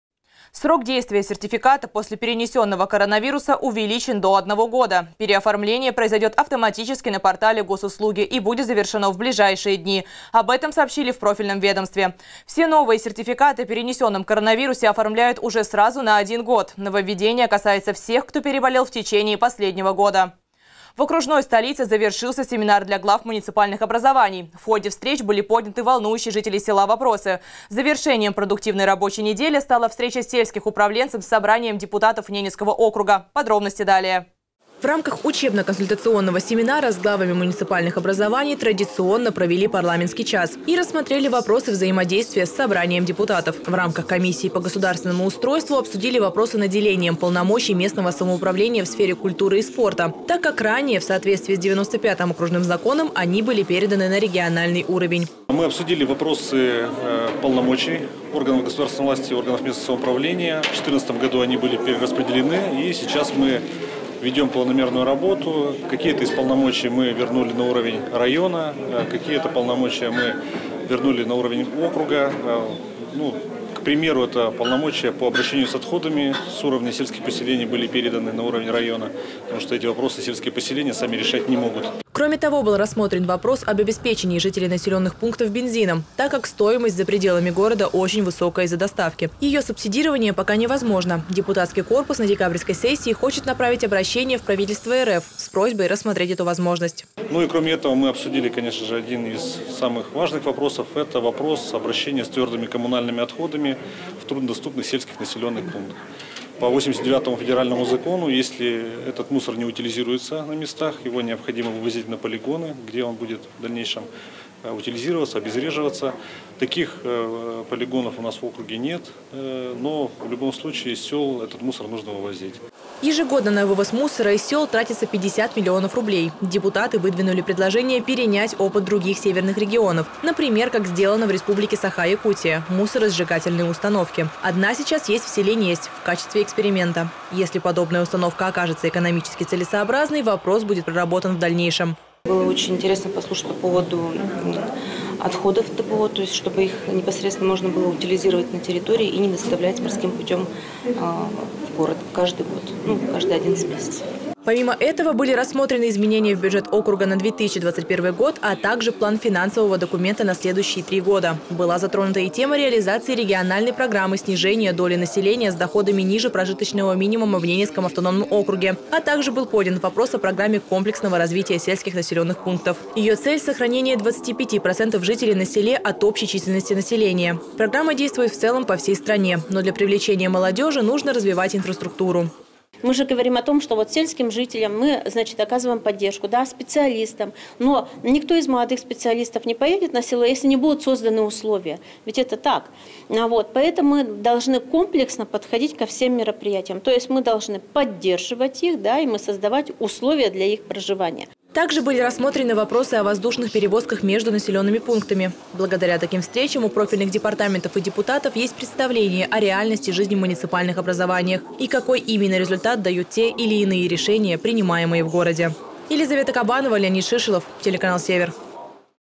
ЗАВЕРШЕНИЕ СЕМИНАРА
В окружной столице завершился семинар для глав муниципальных образований. В ходе встреч были подняты волнующие  жителей села вопросы. Завершением продуктивной рабочей недели стала  встреча сельских управленцев с собранием депутатов Ненецкого округа.